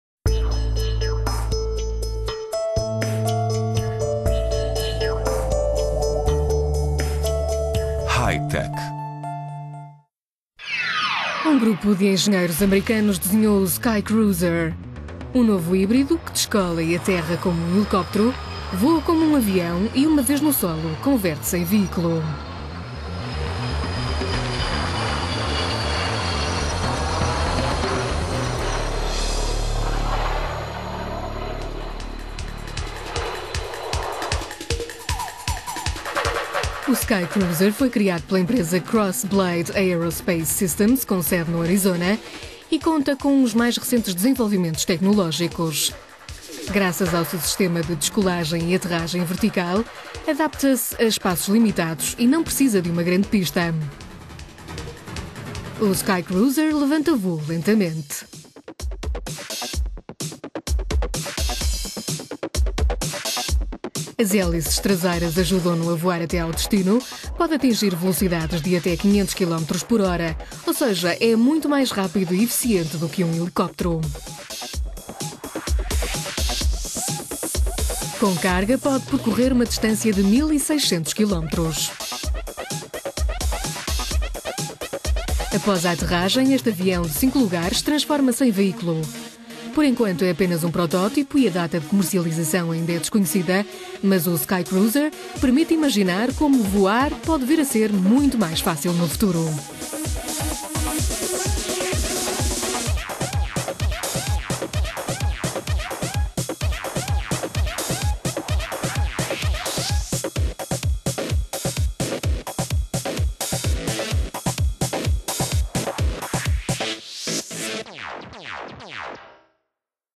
Skycruiser: Avião três em um - hi-tech
Um grupo de engenheiros americanos desenharam o Skycruiser: um novo híbrido que descola e aterra como um helicóptero, voa como um avião, e uma vez no solo, converte-se em veículo. O Skycruiser foi criado pela empresa "Krossblade Aerospace Systems", com sede no Arizona e conta com os mais recentes desenvolvimentos tecnológicos. Graças ao seu sistema de descolagem  e aterragem vertical, adapta-se a espaços limitados e não precisa de uma grande pista. O Skycruiser levanta voo lentamente.